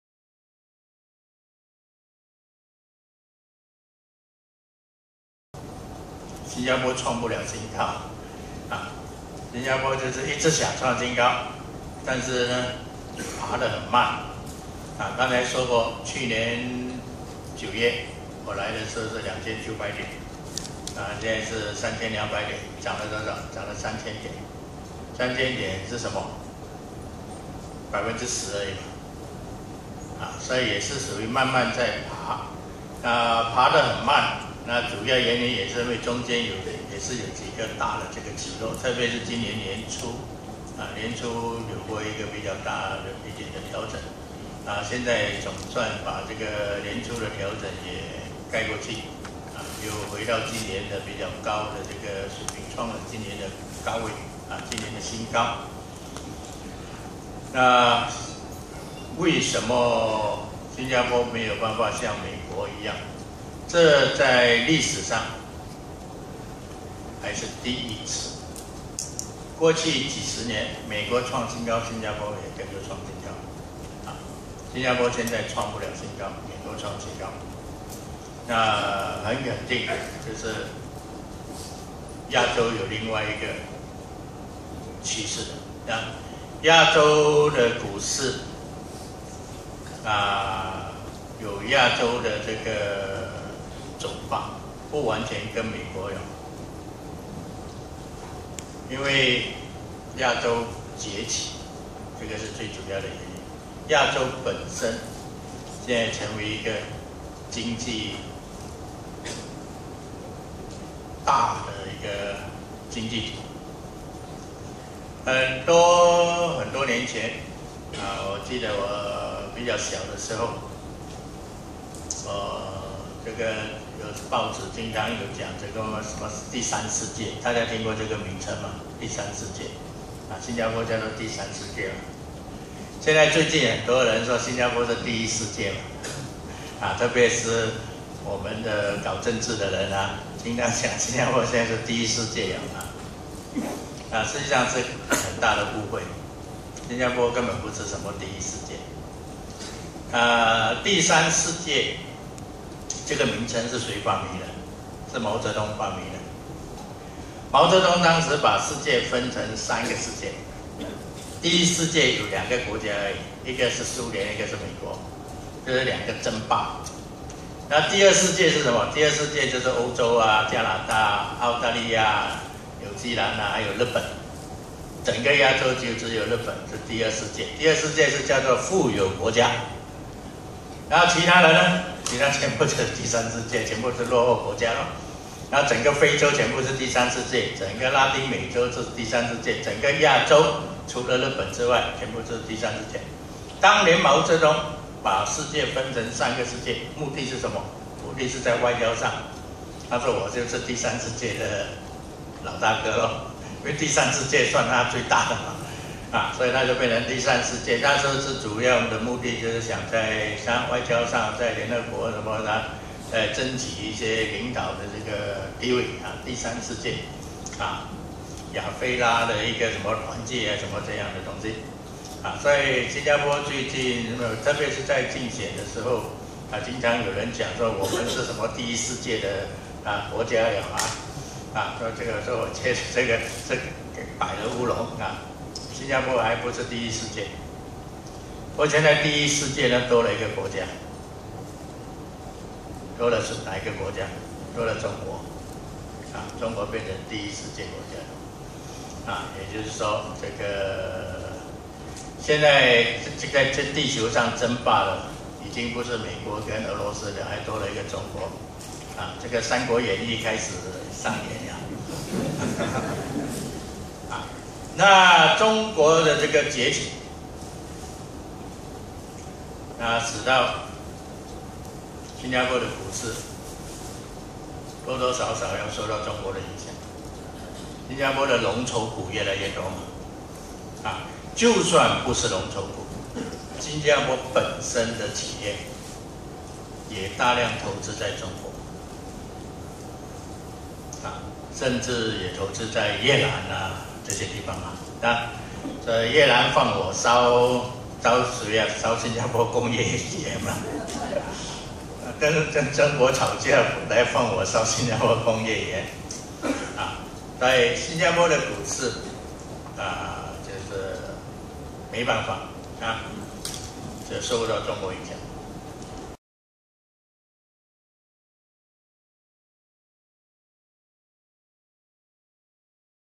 曾渊沧博士开场演讲节录-Part 2—《股市资讯》2014年中股市展望研讨会
《股市资讯》2014年中股市展望研讨会
2014年5月24日；标新局大厦；
曾渊沧博士解释为什么新加坡股市在今年还无法创新高